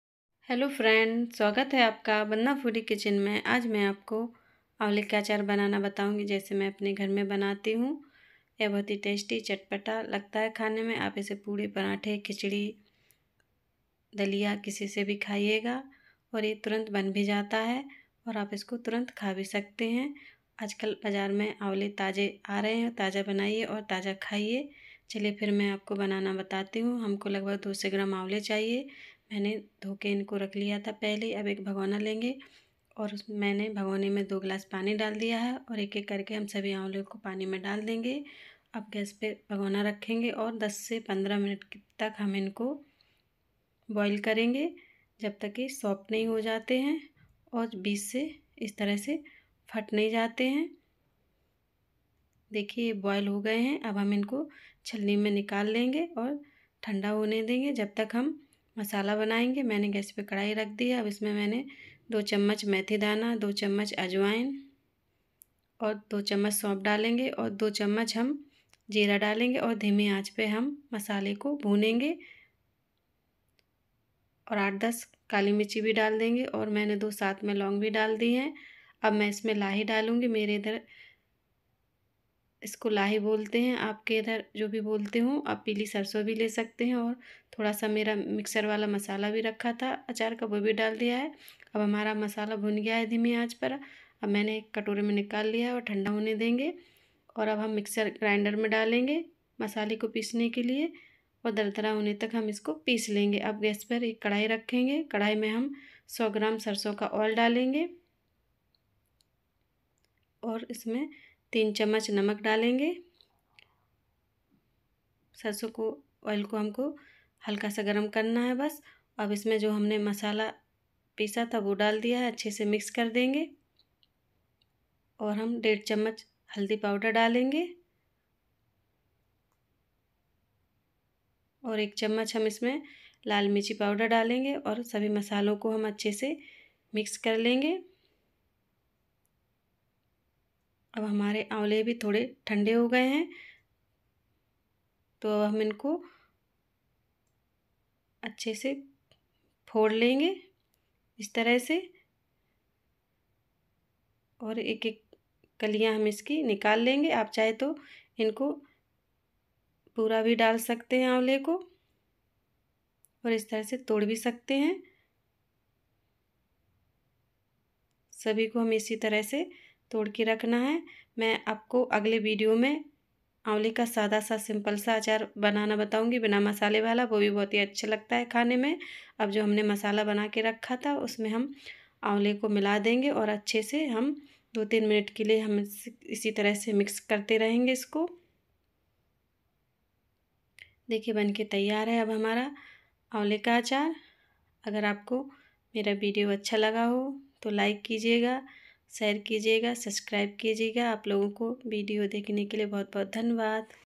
झटपट बनाएं आंवले का अचार और तुरंत खाएं/jhatpat banae aanwale ka Achar aur turant khayen
झटपट बनाएं आंवले का अचार और तुरंत खाएं/jhatpat banae aanwale ka Achar aur turant khayen#achar#murabba#cooking recipe#sweet recipe virat thali#kids Lunch Box idea#lunch#dinner

200g,r aanwala
100 sarsho Oli
2spoon , shop ,ajbain, methi dana, jeera
2 spoon,haldi,1spoon, lal mirchi,3 spoon ,namak,

vandana foodie kitchen